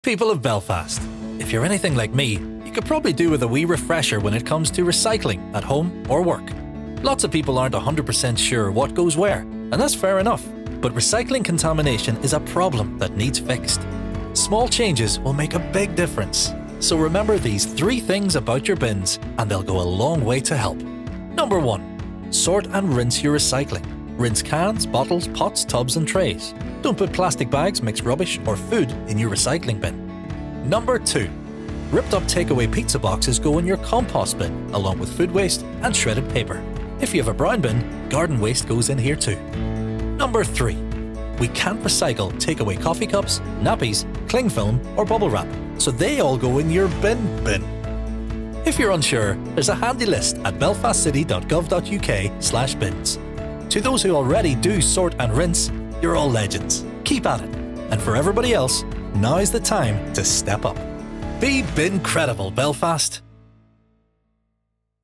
Back to basics recycling
Our Be Bincredible Belfast campaign encourages everyone to keep recycling what you can to support jobs and help the environment. If you aren’t sure where to start, watch our video with three tips on getting the basics right and make sure you’re recycling the right way. for more information and advice on recycling at home